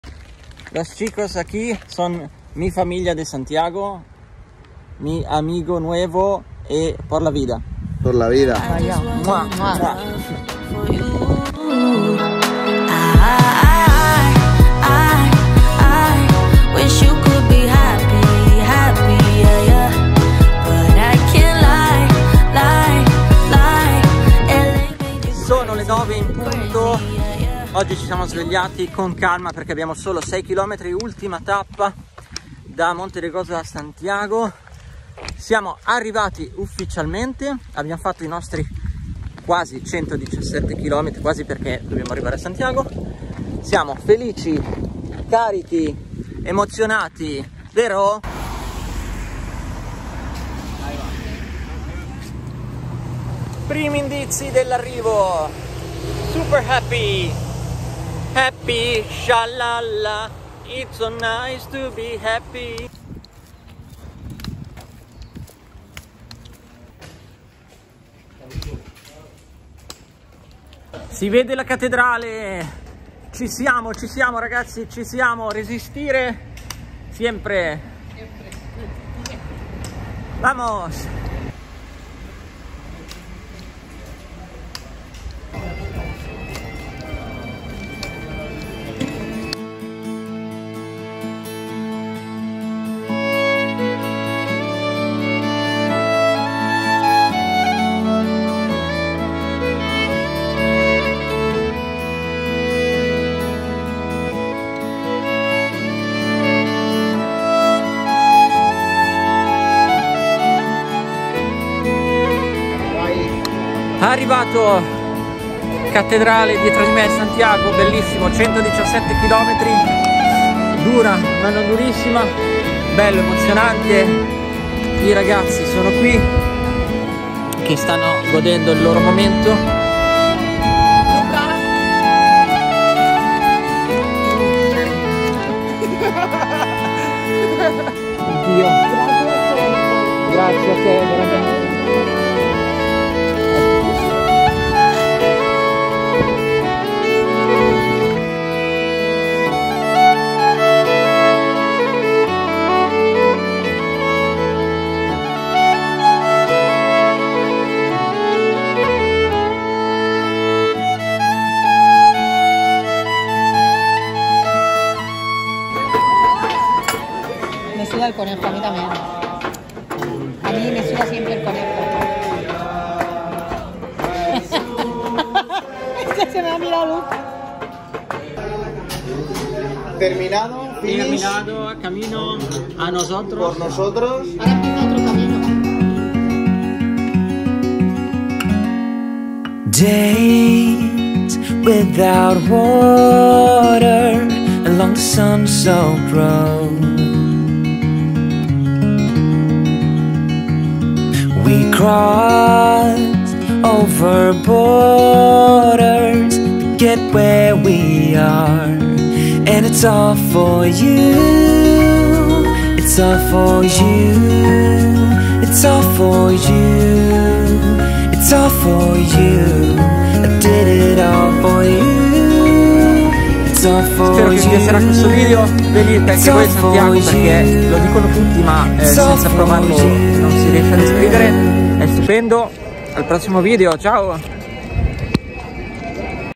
Cammino di Santiago, ultima tappa con pianti e abbracci, arrivati a Santiago
Non volevo partire per il Cammino di Santiago perchè l'idea di non farlo tutto aveva fatto svanire la mia idea di avventura, avevo paura di rimanere deluso, di non emozionarmi e che il numero di passi non fossero sufficienti a rimettere insieme i miei pensieri. Sono partito da Sarria e ho percorso solo 117 km ma ho trovato il mio cammino, la mia dimensione. Non ho sentito il peso dello zaino, la fatica o il fastidio dei vestiti bagnati camminando sotto la pioggia, ho trovato lo spirito del cammino.
Tra i boschi della Galizia e per puro caso ho incontrato due persone fantastiche che sono state la vittoria più grande di questa avventura: Alvaro e Vanessa sono la meta che mi ero prefissato. Partire senza pensarci troppo, senza organizzare e lasciarsi travolgere dagli incontri che questo cammino sa regalare. ero partito da solo ma sul cammino non si è mai soli!
Ultreya et Suseya